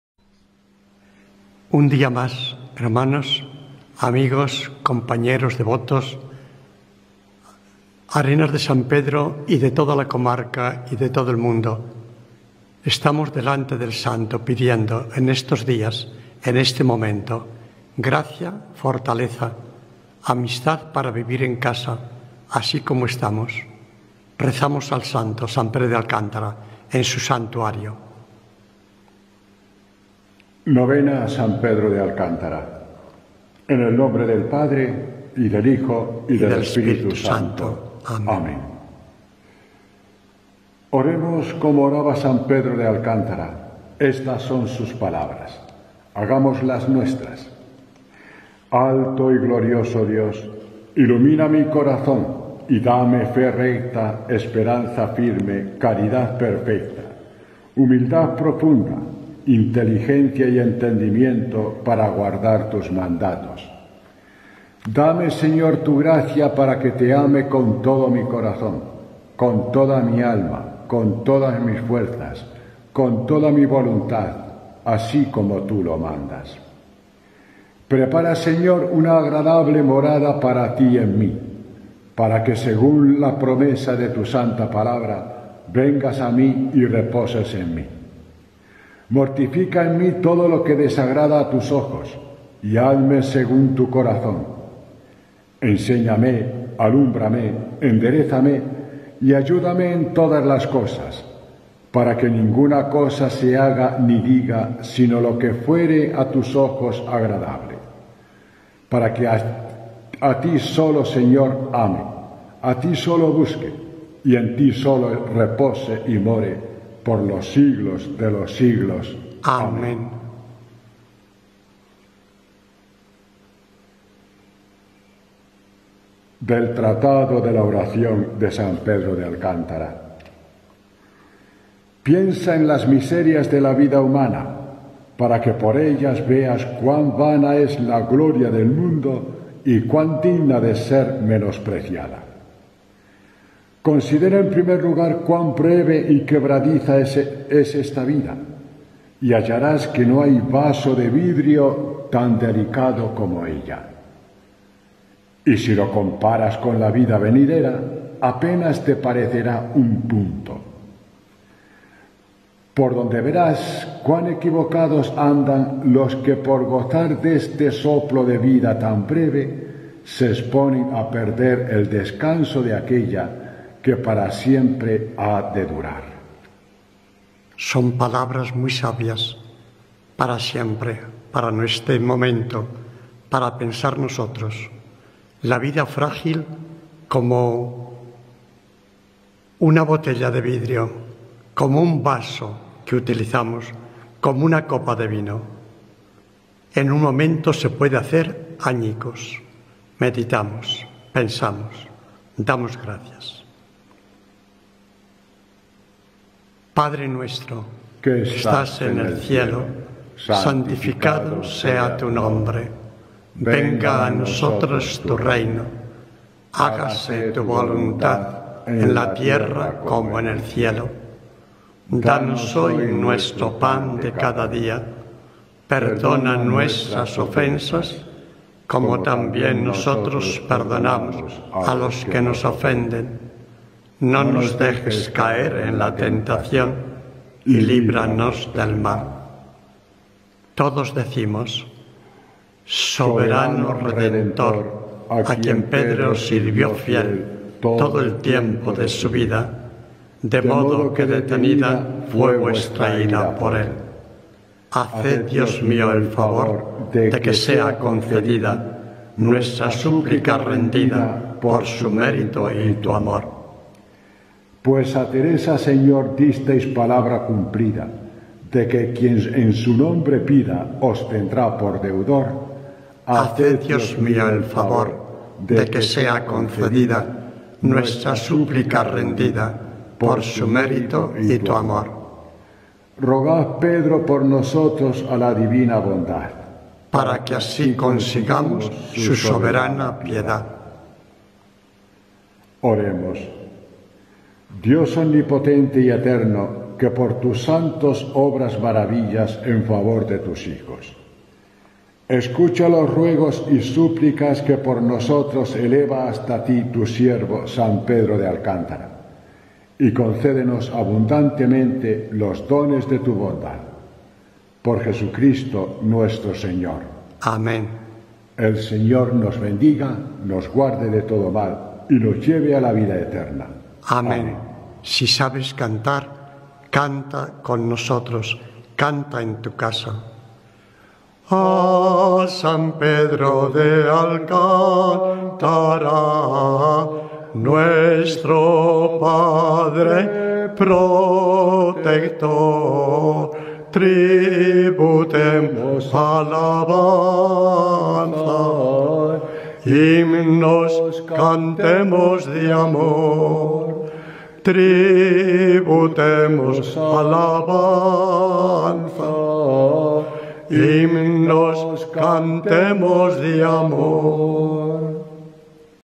SAN PEDRO DE ALCÁNTARA 2. COVID-19. ESTAMOS CON VOSOTROS
SAN PEDRO DE ALCÁNTARA. 2
COVID-19
ESTAMOS CON VOSOTROS.

Segundo día de la Novena a san Pedro de Alcántara .

Desde el Santuario de san Pedro de Alcántara, en Arenas de san Pedro (Ávila), España, nos sentimos unidos a todas vuestras familias, a todas las personas. Estamos en un momento de reflexión, de dificultad, de ordenar muchas cosas en nuestras vidas, dentro de los espacios de la casa, del orden que se impone para que todo sea más fácil y menos pesado.
Utilizamos el tiempo, que nos viene ahora tan largo, para rezar, hablar, leer, hacer un programa mínimo de juego, de distracción, de ayuda, de estudio, de atención y diversión.
San Pedro de Alcántara nos ayude en todo.

Covid-19. El coronavirus galopa .

Bendiciones desde el Santuario. Os recordamos en la oración y seguimos las noticias. Que todo nos sirva para bien, y el bien se posesione de nuestras casas.

Dios es nuestro Sumo Bien, Total Bien.

NB.- Para pasar este vídeo se han necesitado más de 14 horas, pues estamos en el monte.

Libro: Lecturas y Canciones. Humanidad en el Universo